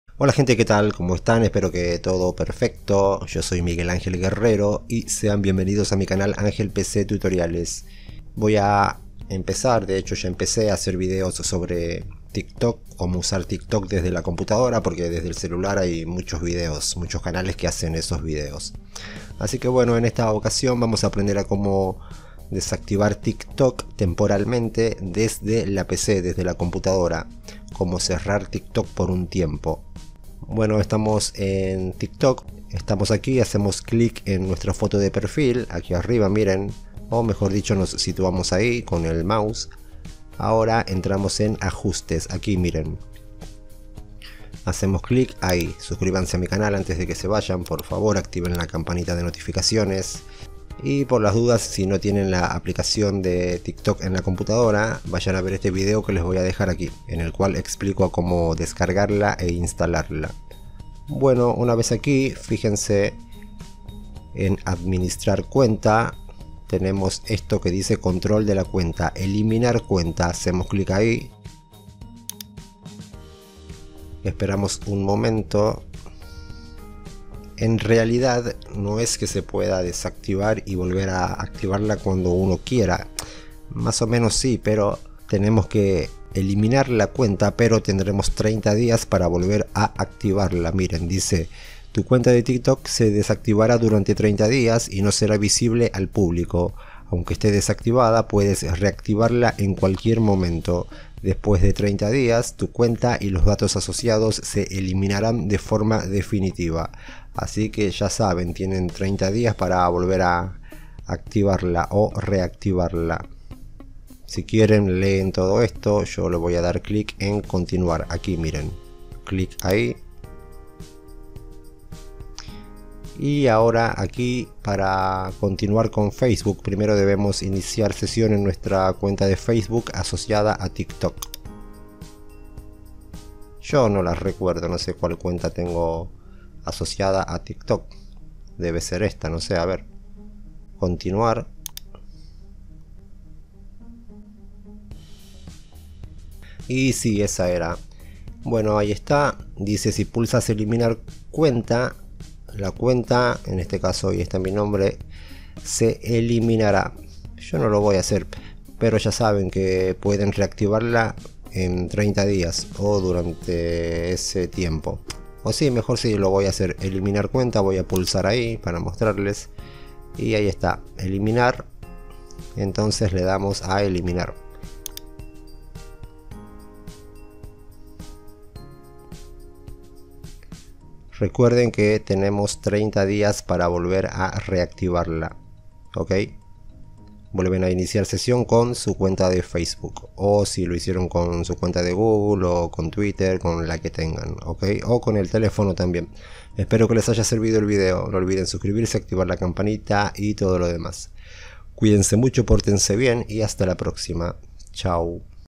COMO DESACTIVAR TIKTOK TEMPORALMENTE DESDE LA PC 🚀 Cerrar Cuenta Por Un Tiempo 💥 ACTUALIZADO
Cómo desactivar TikTok temporalmente desde la PC. Explico cómo desactivar o cerrar tu cuenta de TikTok por un tiempo en la computadora, ya sea desde la aplicación de TikTok para ordenadores, o desde la página web. Sea el motivo que sea por el cual quieras desactivar tu cuenta de TikTok temporalmente, en este video tutorial aprenderás la manera correcta de hacerlo desde tu PC. Y ya sabes que, si deseas volver a activar o reactivar tu cuenta de TikTok, puedes hacerlo sin problemas. Por favor, mira el video de principio a fin porque explico cosas muy importantes a lo largo del mismo.